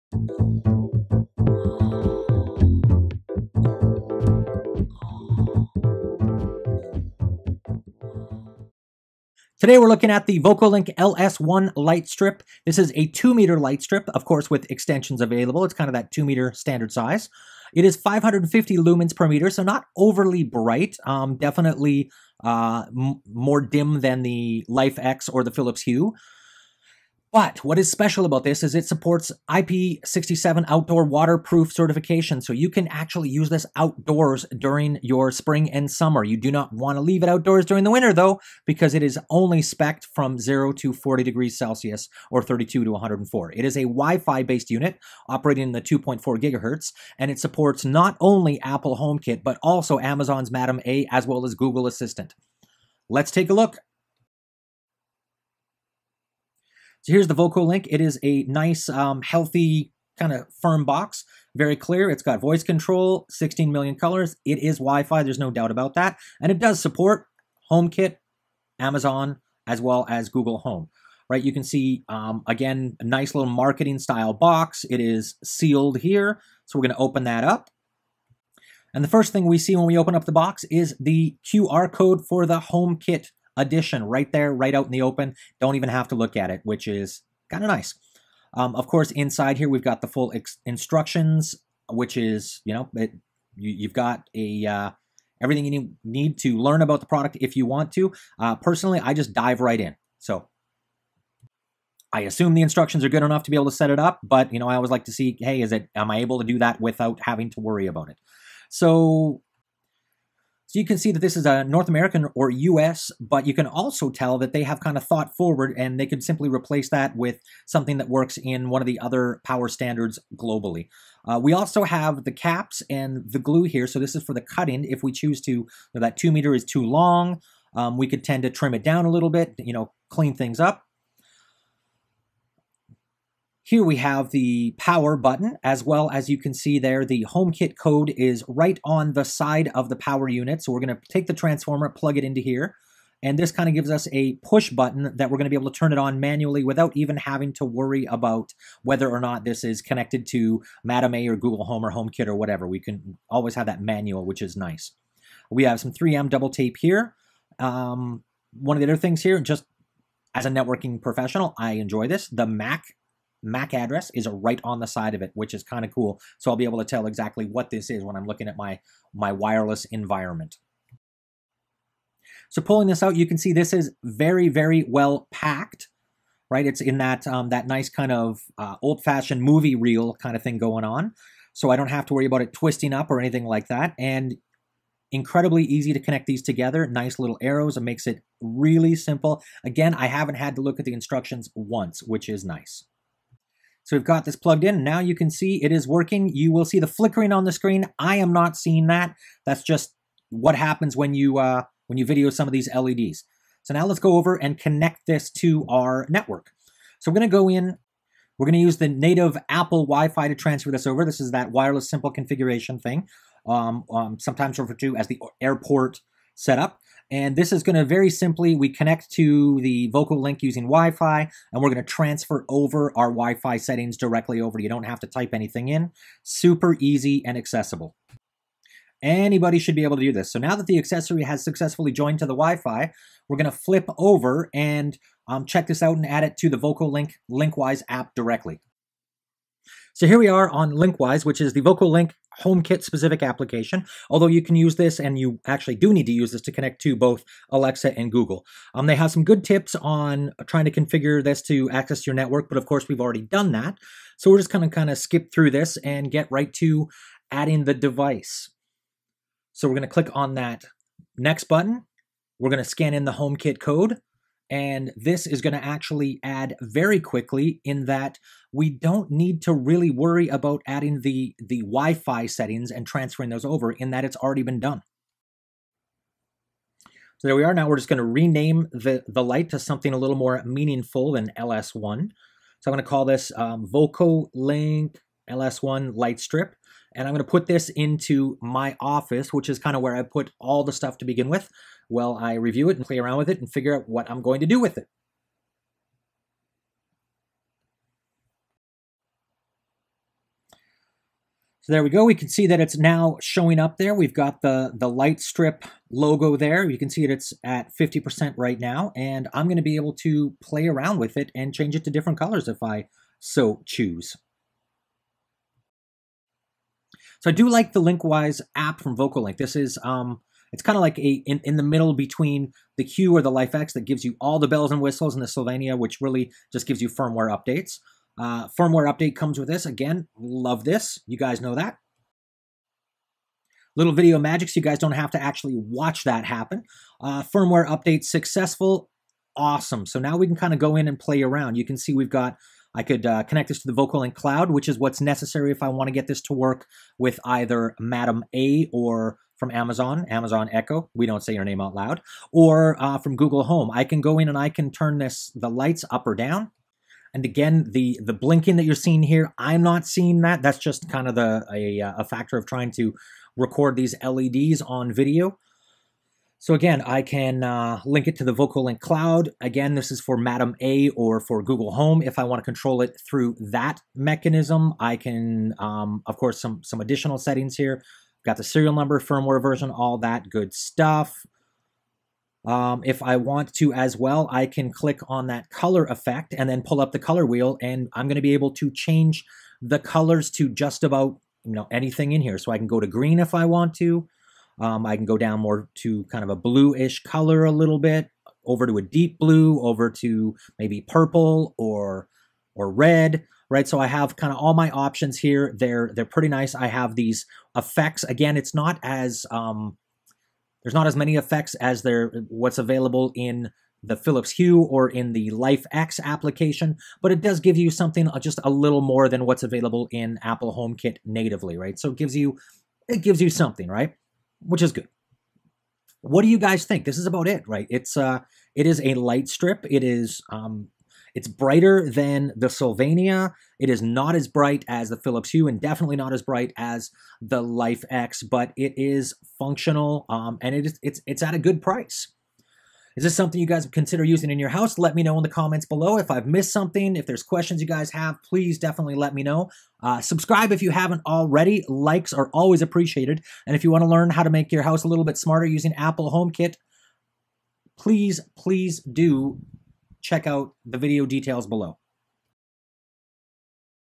Vocolinc LS1 Light Strip (Apple Homekit)
No smarthouse is complete without at least one smart LED light strip and the Vocolinc LS1 light strip allows you to extend that outdoors with it's IP67 waterproof rating!  Compatible with Homekit, Amazon Alexa, as well as GoogleHome this light strip is a great addition to your SmartHome and will really brighten up your garden parties with whatever color mood lighting is appropriate for the occasion.

Vocolinc LS1